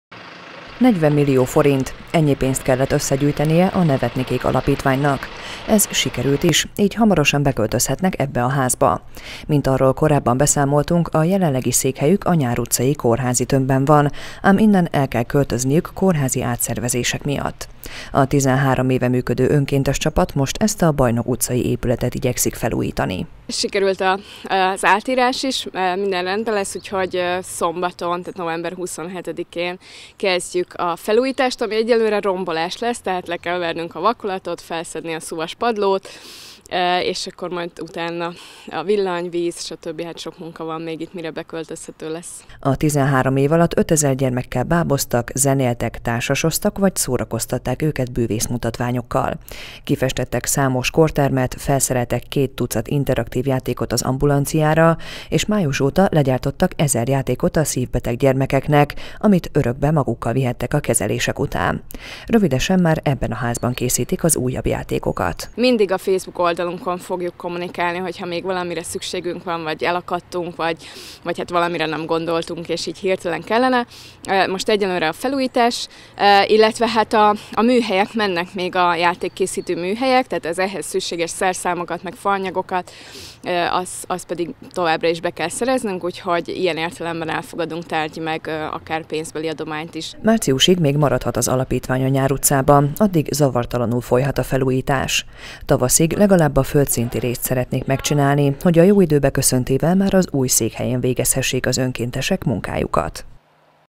Sikerült összegyűjteni a 40 millió forintot, így új helyre költözhet Nevetnikék Alapítvány
Sikerült összegyűjteni azt a 40 millió forintot, ami egy ház vásárlásához kellett a Nevetnikék Alapítványnak. A 13 éve működő pécsi szervezet ugyanis csak márciusig maradhat jelenlegi telephelyén, a Nyár utcában. A napokban meg is kezdik a felújítást új székhelyükön, a Bajnok utcában.